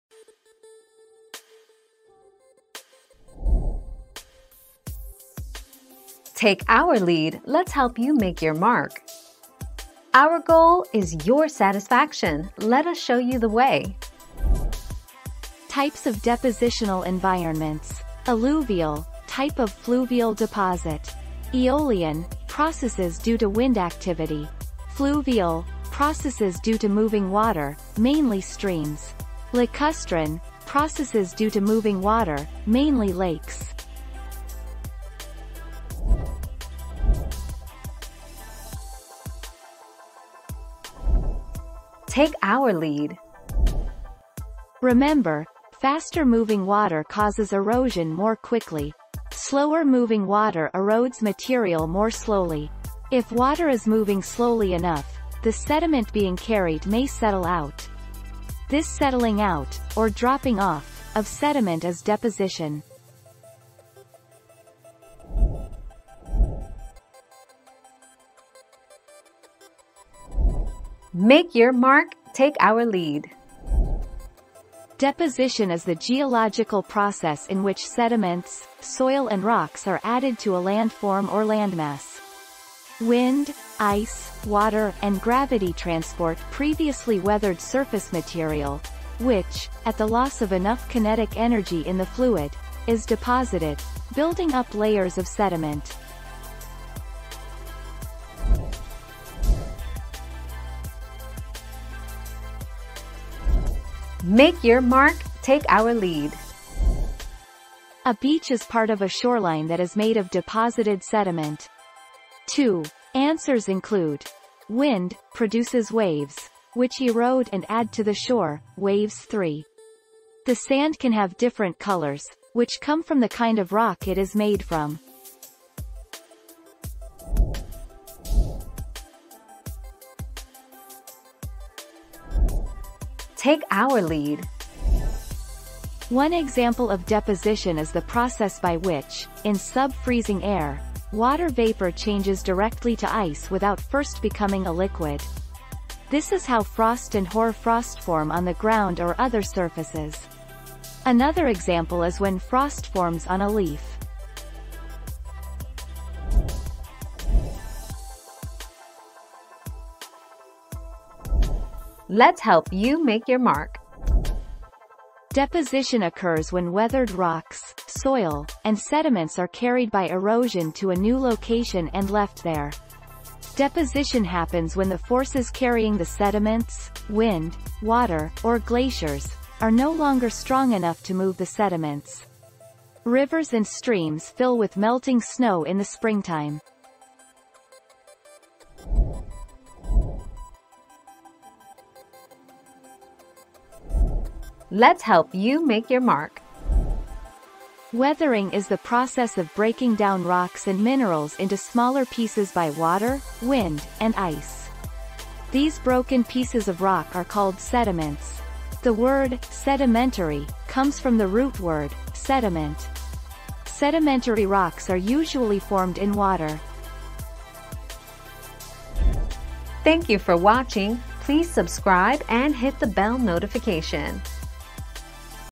What are 3 types of deposition?
00:00 - What are 3 types of deposition?
00:37 - Is deposition fast or slow?
01:07 - What are the main causes of deposition?
01:40 - Is a beach erosion or deposition?
02:08 - What's an example of deposition?
02:37 - How is deposition caused?
03:09 - What are broken down pieces of rock called?

Laura S. Harris (2021, May 9.) What are 3 types of deposition?
    AskAbout.video/articles/What-are-3-types-of-deposition-242810